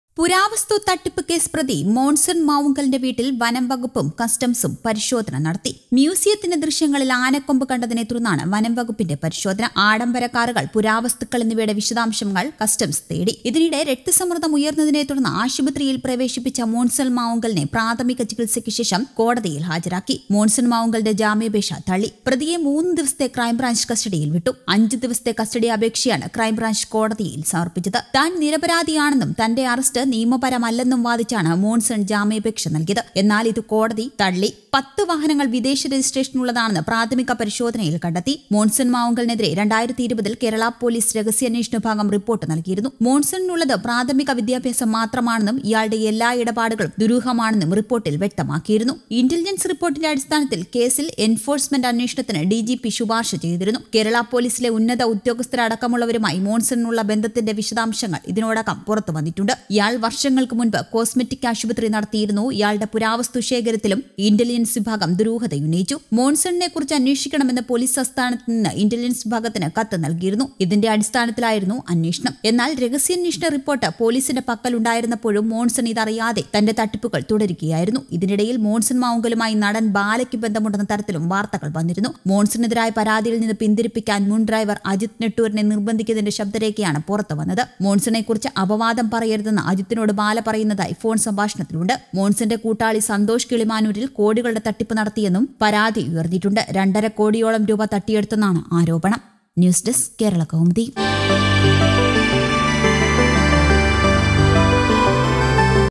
Customs and forest dept raid Monson Mavunkal's house in Kaloor | Keralakaumudi
പുരാവസ്തു തട്ടിപ്പുകേസ് പ്രതി മോന്‍സന്‍ മാവുങ്കലിന്റെ വീട്ടില്‍ വനംവകുപ്പും കസ്റ്റസും പരിശോധന നടത്തുന്നു. മ്യൂസിയത്തിന്റെ ദൃശ്യങ്ങളില്‍ ആനക്കൊമ്പ് കണ്ടതിനെ തുടര്‍ന്നാണ് വനംവകുപ്പിന്റെ പരിശോധന. ആഡംബരക്കാറുകള്‍, പുരാവസ്തുക്കള്‍ എന്നിവയുടെ വിശദാംശങ്ങള്‍ കസ്റ്റംസ് തേടു. ഇതിനിെട രക്തസമ്മര്‍ദം ഉയര്‍ന്നതിനെ തുടര്‍ന്ന് ആശുപത്രിയില്‍ പ്രവേശിപ്പിച്ച മോന്‍സന്‍ മാവുങ്കലിനെ പ്രാഥമിക ചികില്‍സയ്ക്കുശേഷം കോടതിയില്‍ ഹാജരാക്കി. മോണ്‍സണ്‍ മാവുങ്കലിന്റെ ജാമ്യപേക്ഷ തള്ളി. പ്രതിയെ മൂന്നു ദിവസത്തെ ക്രൈംബ്രാഞ്ച് കസ്റ്റഡിയില്‍ വിട്ടു. അഞ്ചു ദിവസത്തെ കസ്റ്റഡി അപേക്ഷയാണ് ക്രൈംബ്രാഞ്ച് കോടതിയില്‍ സമര്‍പ്പിച്ചത്. താന്‍ നിരപരാധിയാണെന്നും തന്റെ അറസ്റ്റ് നിയമപരമല്ലെന്നും വാദിച്ചാണ് മോണ്‍സണ്‍ ജാമ്യപേക്ഷ നല്‍കിയത്. എന്നാല്‍ ഇത് കോടതി തുകയായിരുന്നു.പത്ത് വാഹനങ്ങള്‍ വിദേശ രജിസ്‌ട്രേഷനിലുള്ളതാണെന്ന് പ്രാഥമിക പരിശോധനയില്‍ കണ്ടെത്തി.മോന്‍സണ്‍ മാവുങ്കലിനെതിരെ 2020 ല്‍ കേരള പൊലീസ് രഹസ്യാന്വേഷണ വിഭാഗം റിപ്പോര്‍ട്ട് നല്‍കിയിരുന്നു. മോന്‍സണിനുള്ളത് പ്രാഥമിക വിദ്യാഭ്യാസം മാത്രമാണെന്നും ഇയാളുടെ എല്ലാ ഇടപാടുകളും ദുരൂഹമാണെന്നും റിപ്പോര്‍ട്ടില്‍ വ്യക്തമാക്കിയിരുന്നു.